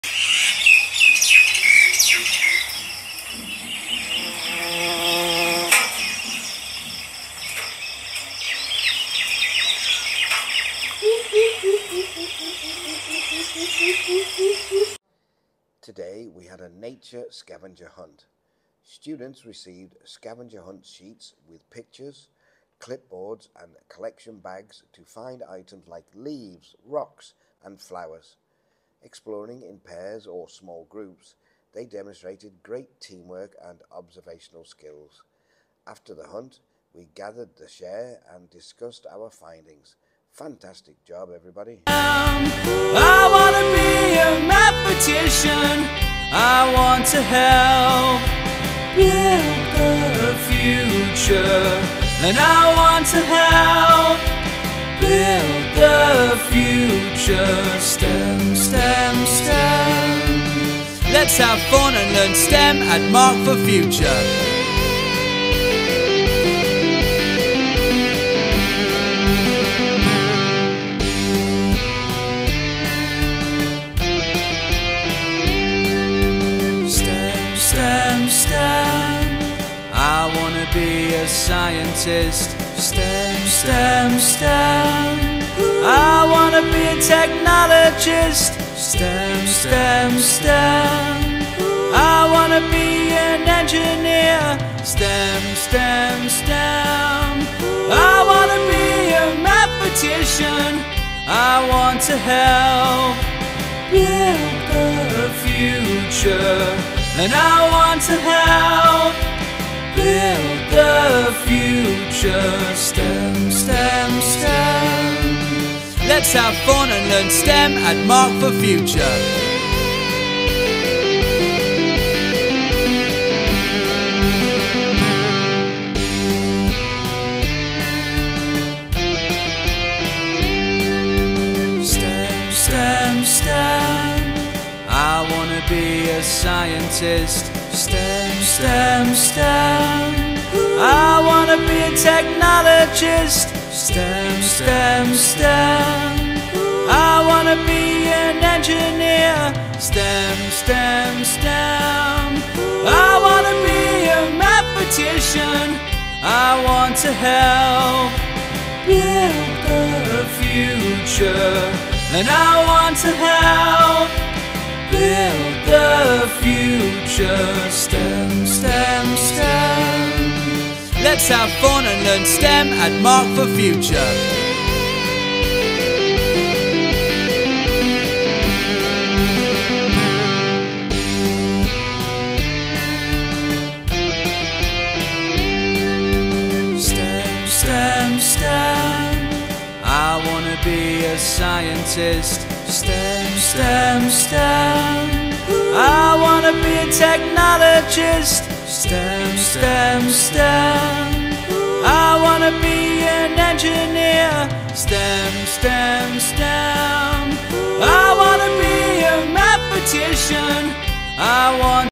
A Nature Scavenger Hunt
STEM DAY❤️
Today, we had a nature scavenger hunt. Students received scavenger hunt sheets with pictures, clipboards, and collection bags to find items like leaves, rocks, and flowers. Exploring in pairs or small groups, they demonstrated great teamwork and observational skills. After the hunt, we gathered to share and discuss our findings 🌿🔍. Fantastic job, everyone!